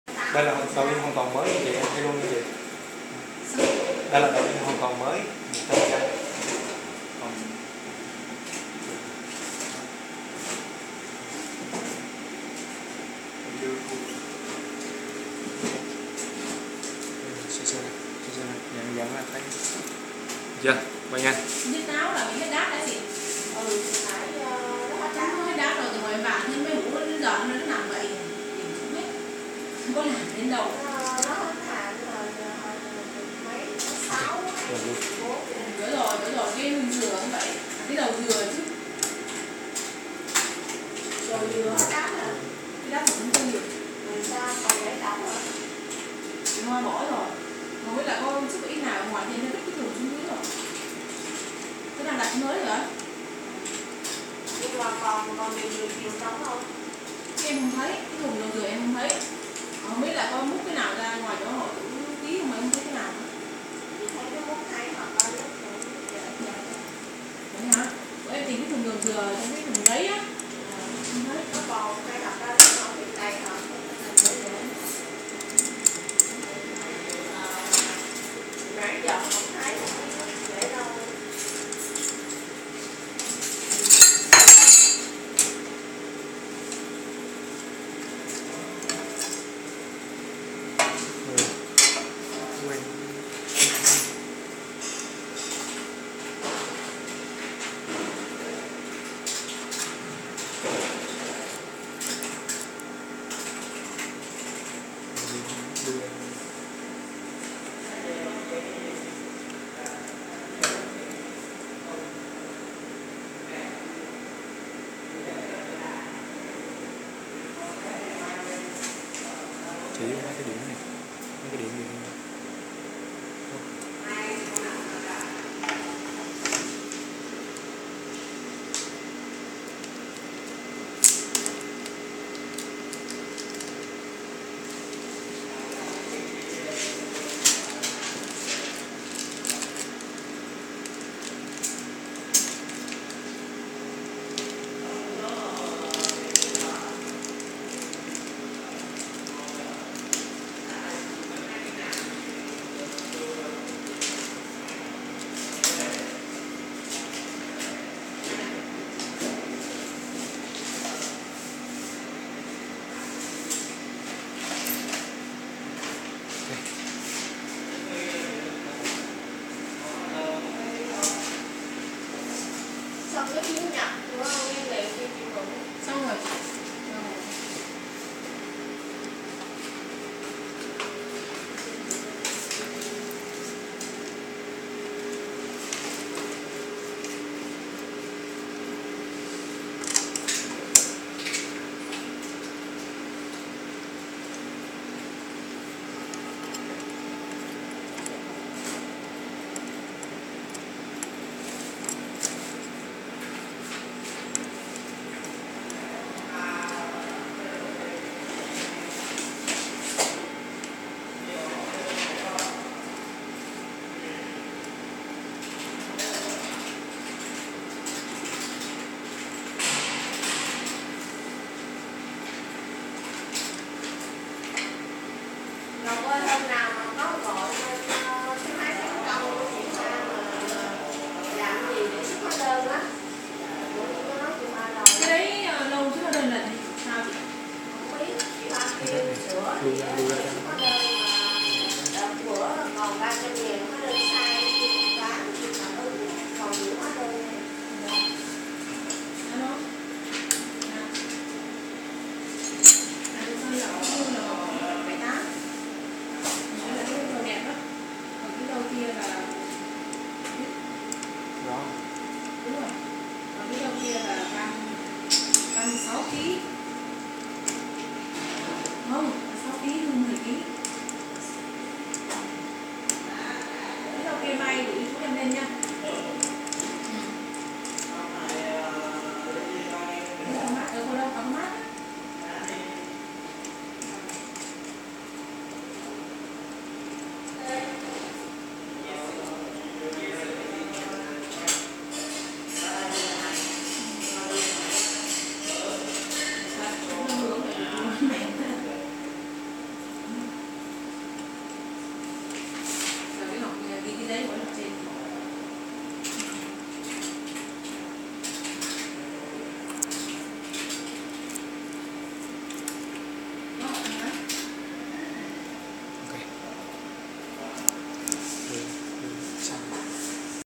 Hướng dẫn lắp đặt đầu in Máy in mã vạch Ring 4012PLM+ | Công Ty Thế Giới Mã Vạch
+ Video sau sẽ hướng dẫn đến bạn cách lắp đặt đầu in Máy in mã vạch Ring 4012PLM+ sao cho đúng và hạn chế gặp phải trình trạng đầu in bị xước trong quá trình lắp.
+ Máy in mã vạch Ring 4012PLM+ có xuất xứ 100% từ Nhật Bản. Hiệu suất in công nghiệp mạnh mẽ, chất lượng in sắc nét, đẹp mắt. Đặc biệt, máy có thể in được trên nhiều loại chất liệu tem nhãn khác nhau, thậm chí là cả tem nhãn vải.